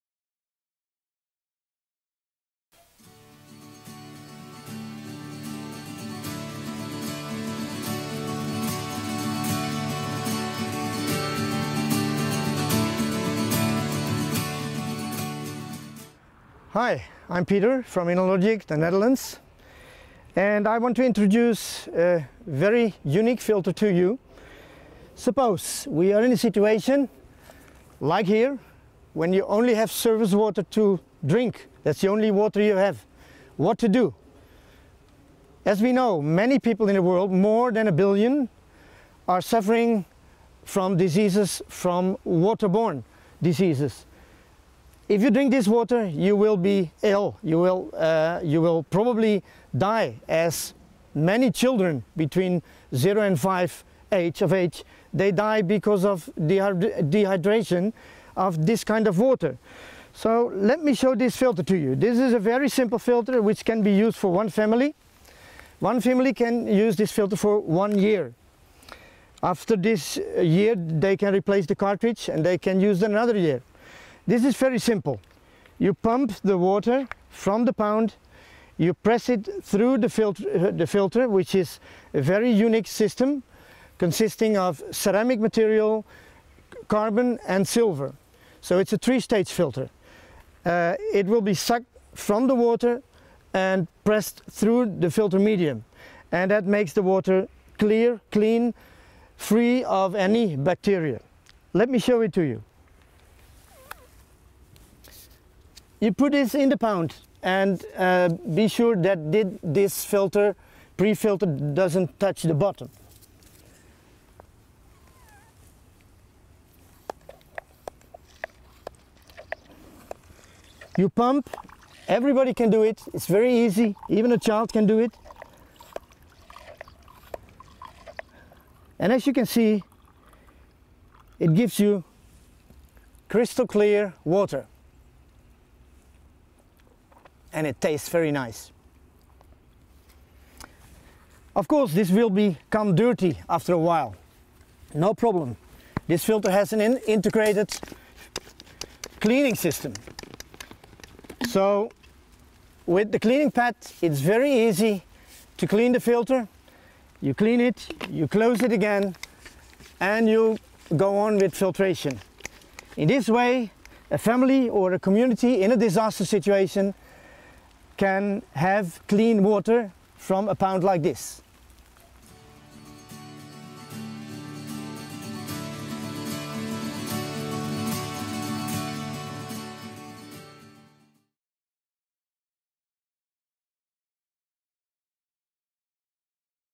CCS Mobile water drinking filter (English version II)
This promovideo shows you how the advanced technology of the Travel Mate waterfilter works. In 3 minutes you will know everything there is to know about this great system. Enjoy!